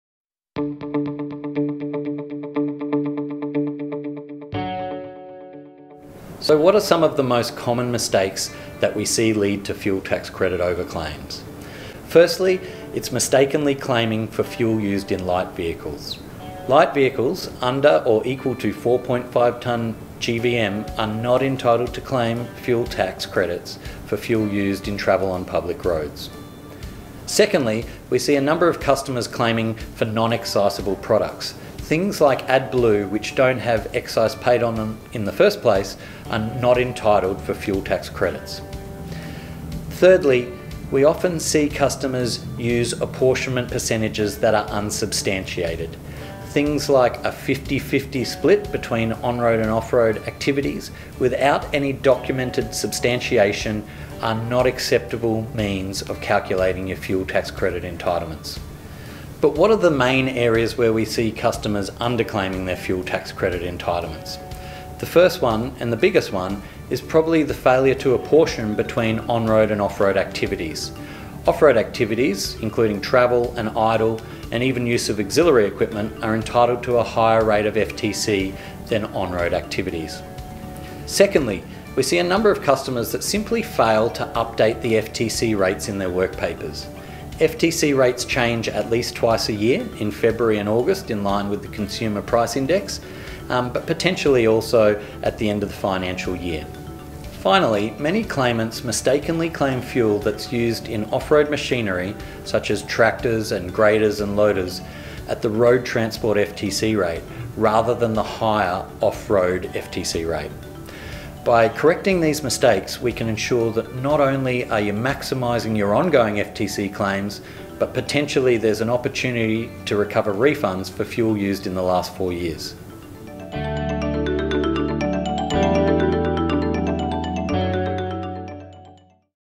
FTC Manager, Common Mistakes & Missing Opportunities
So what are the common mistakes and missing opportunities when it comes to claiming fuel tax credits? Tune in to Peter Perich, director of PPM Tax & Legal discuss the top 3 mistakes followed by the top 3 opportunities that everyone should know about.